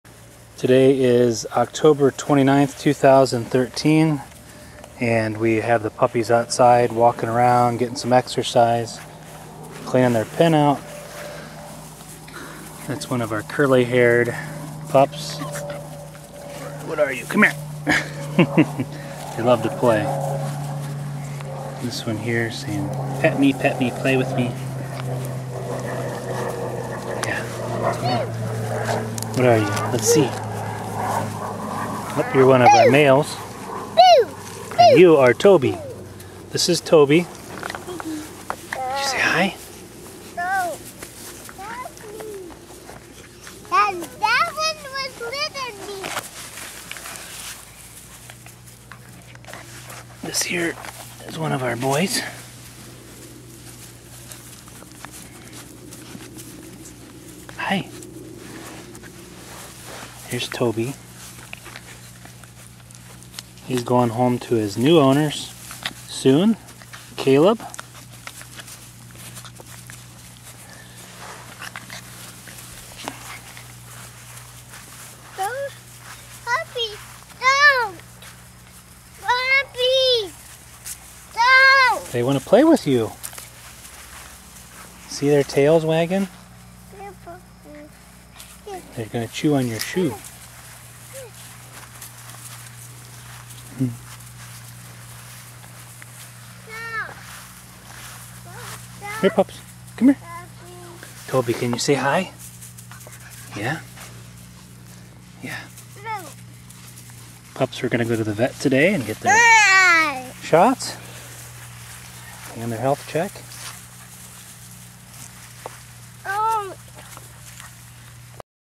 Goldendoodle Pups For Sale - 6 Weeks old
Goldendoodle pups playing outside.  for more information.